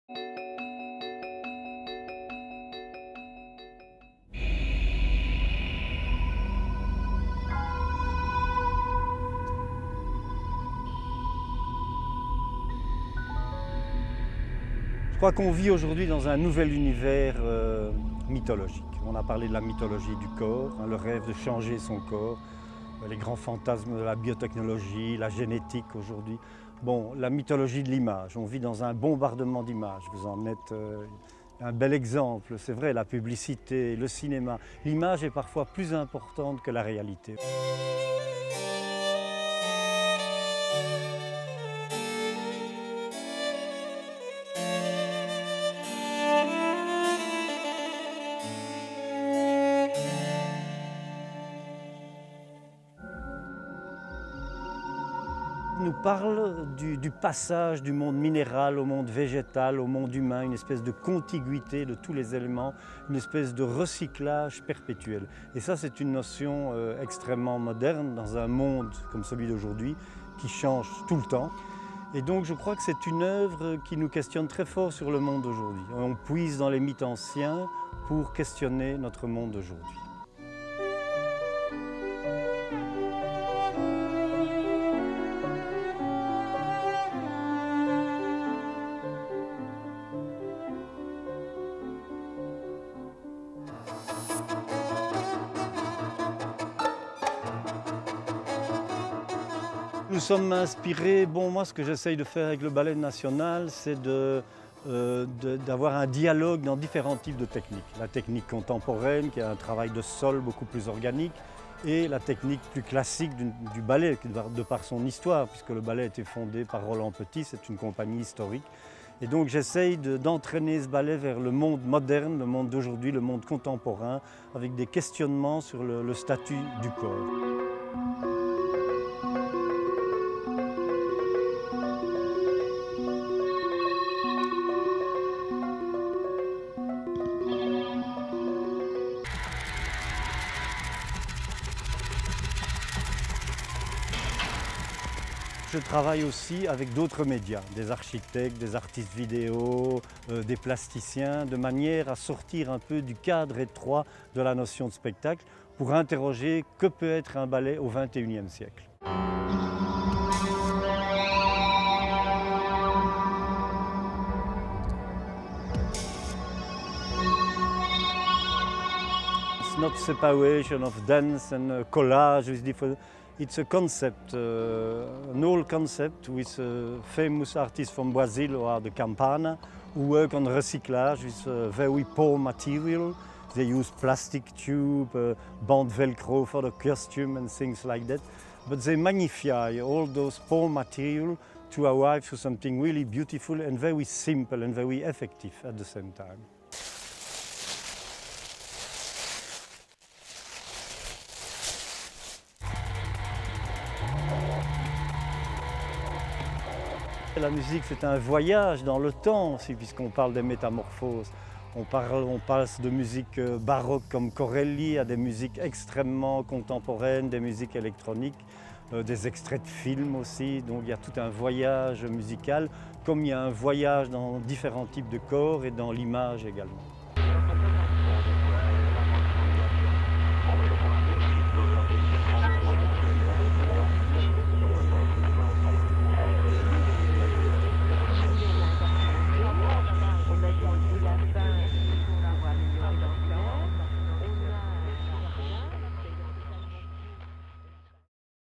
El Ballet Nacional de Marsella presenta "Métamorphoses" en el Teatro Romano
Momentos antes de la primera función del Ballet Nacional de Marsella en la edición 2013 de Itálica Festival Internacional de Danza, el 16 de julio en el Teatro Romano, Frédéric Flamand nos habla sobre "Métamorphoses" y nos da las principales claves para entender el momento actual de esta compañía de prestigio internacional.